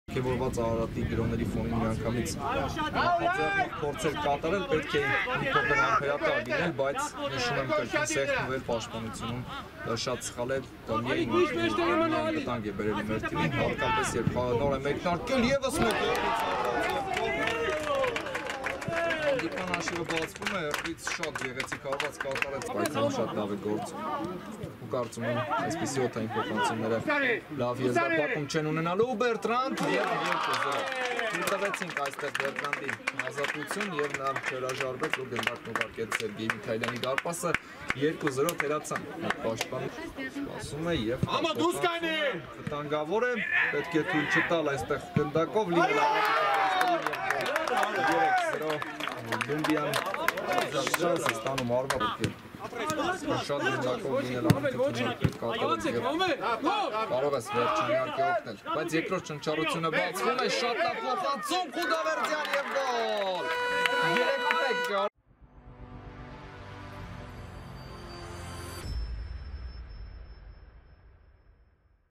Ararat-2 - Pyunik Academy 3:1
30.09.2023, Armenia, First League, Round 10
Goals: Erik Azizyan 11, Bertrand Mani Junior 37, Kalifala Doumbia 60 - Zaven Khudaverdyan 89